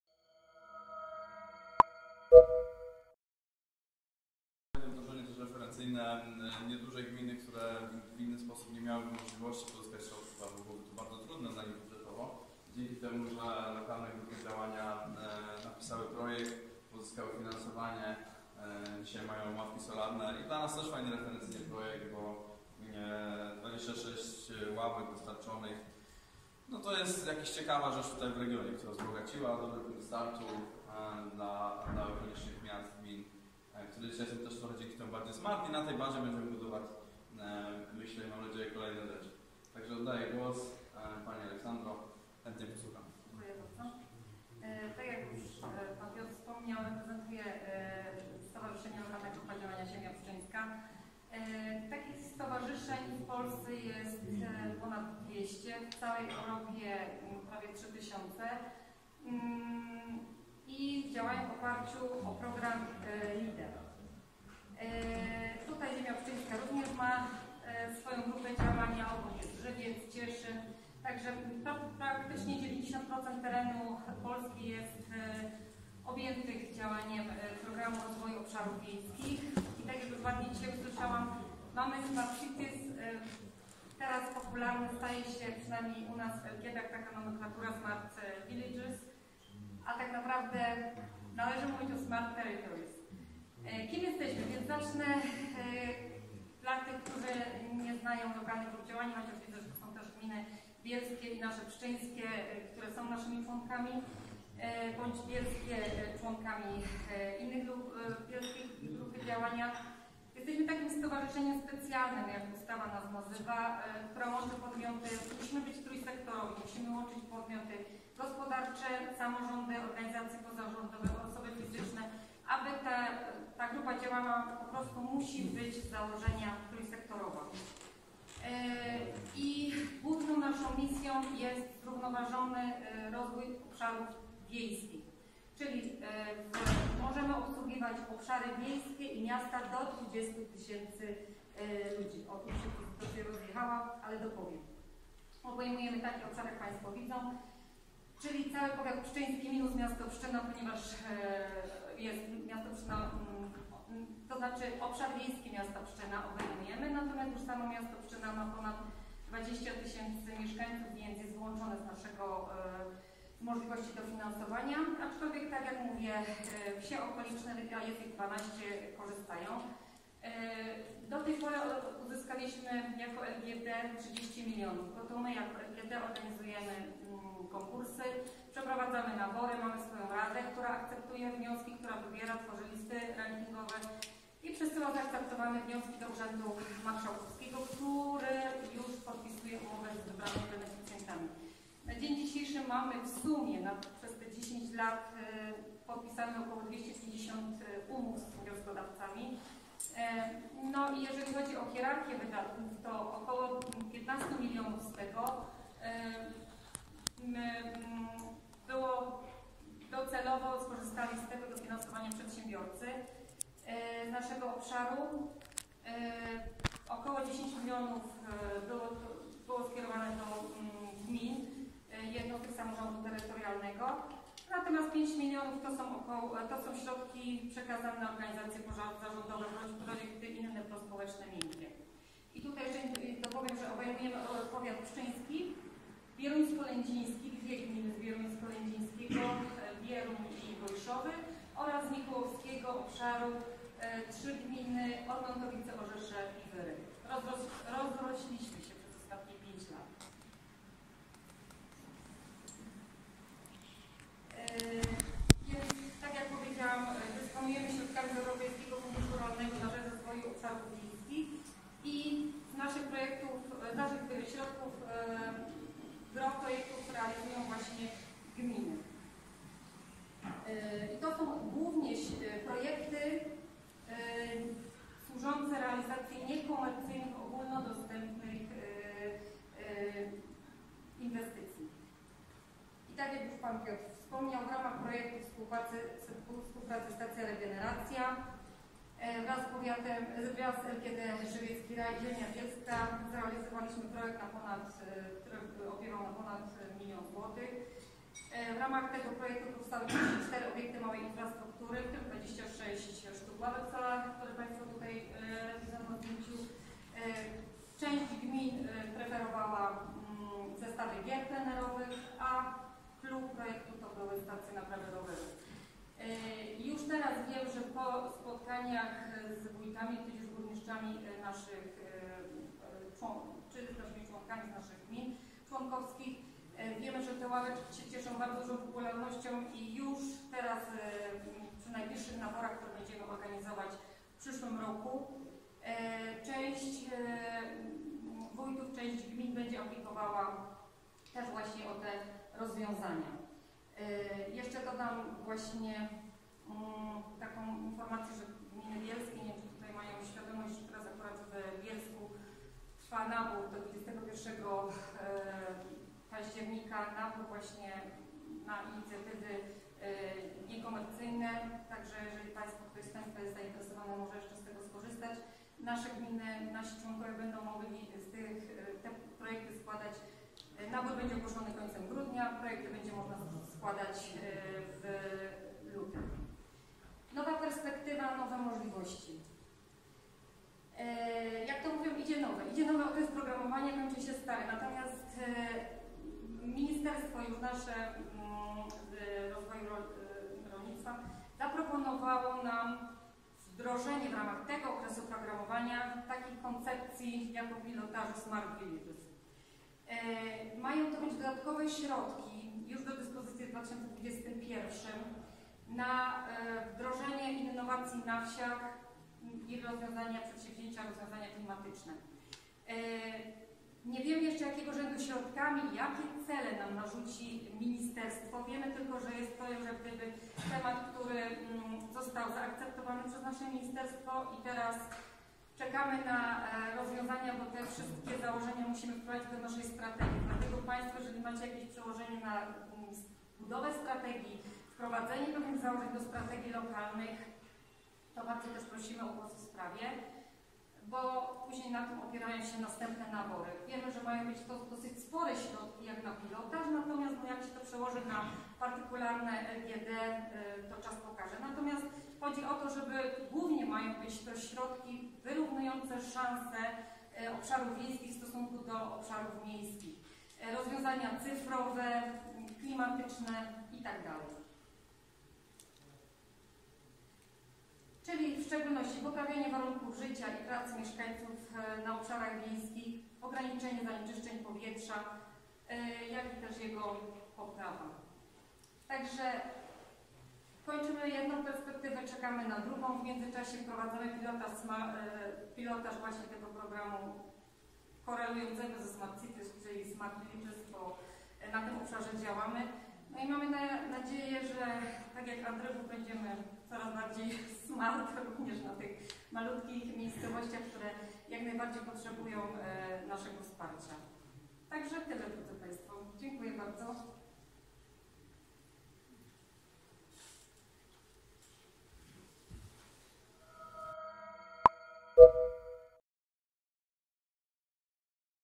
Jak finansować innowacje w gminach? - Konferencja Smart City, Bielsko-Biała
Prezentacja "Jak finansować innowacje w gminach?" przedstawiona przez Stowarzyszenie Lokalna Grupa Działania "Ziemia Pszczyńska", w ramach II edycji konferencji Smart City w Bielsku-Białej organizowanej przez SEEDiA.